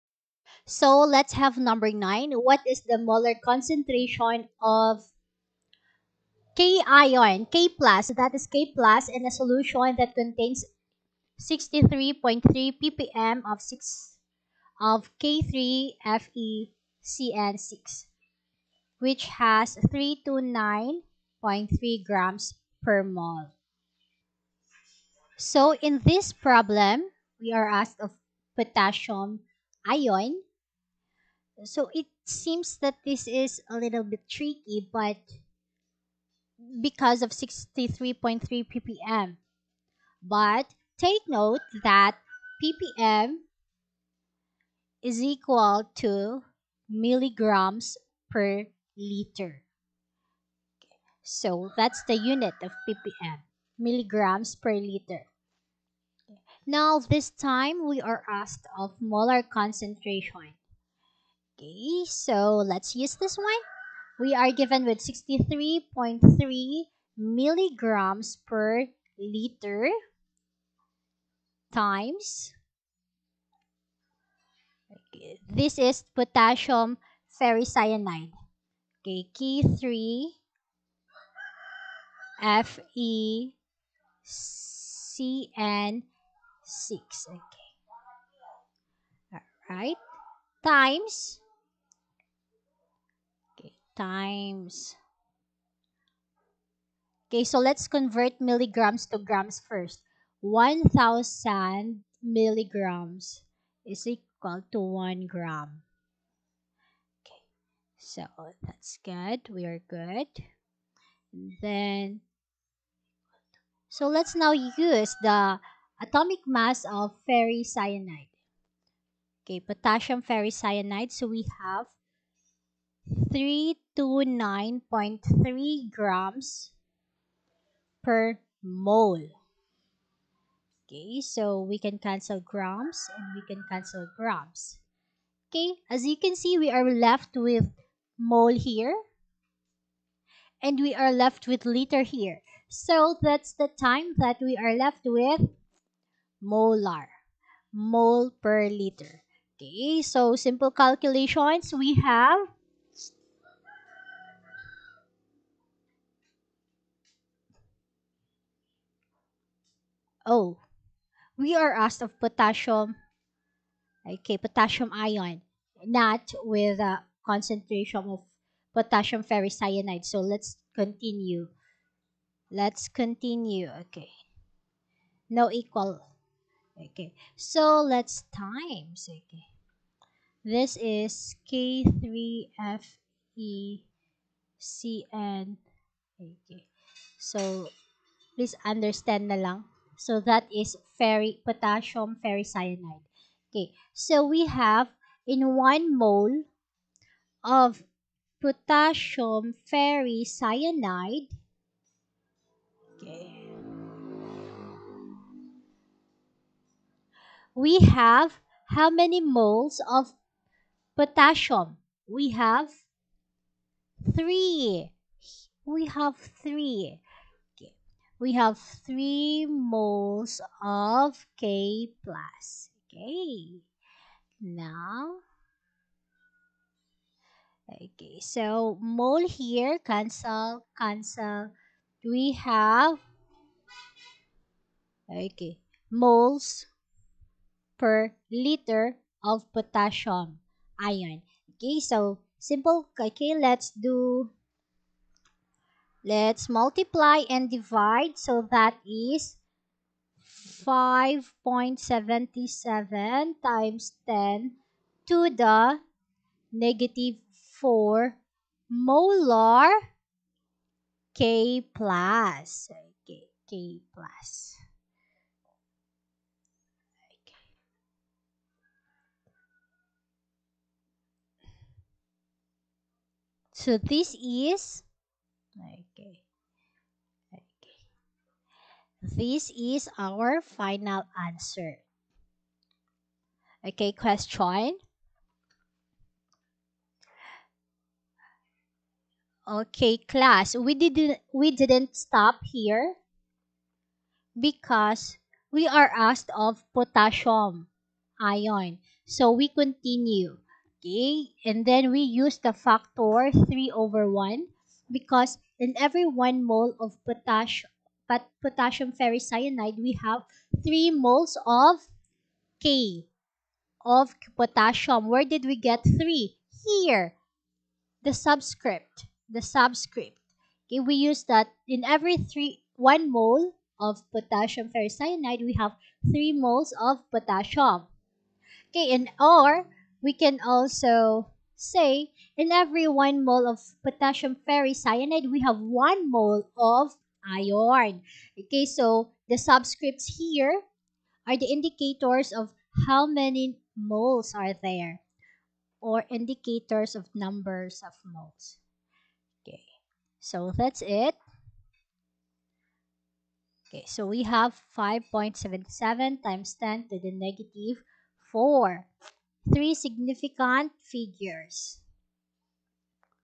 Solving Concentration given ppm
What is the molar concentration of K+ in a solution that contains 63.3 ppm of K3Fe(CN)6 (329.3 g/mol)?